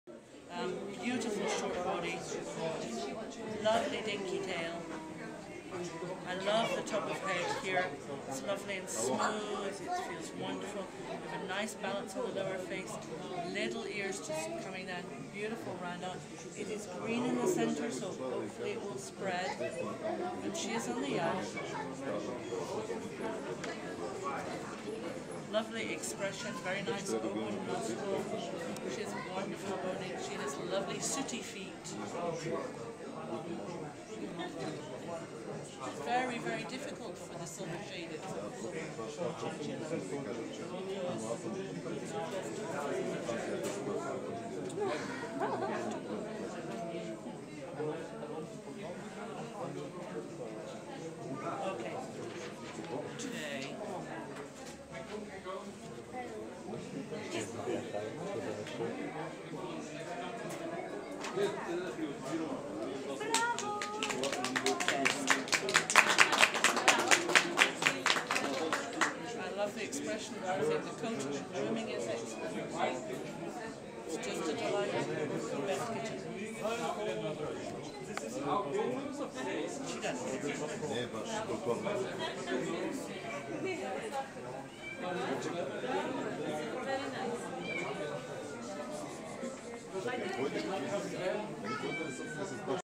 Stefilla Queen of Catwalk's first TICA-final
Stefilla Queen of Catwalk - 5 month old black silver chinchilla persian girl on her first TICA Show in Sofia (Bulgaria) did Best Allbreed Kitten in Phillipa Holmes final
I'm sooo proud of her!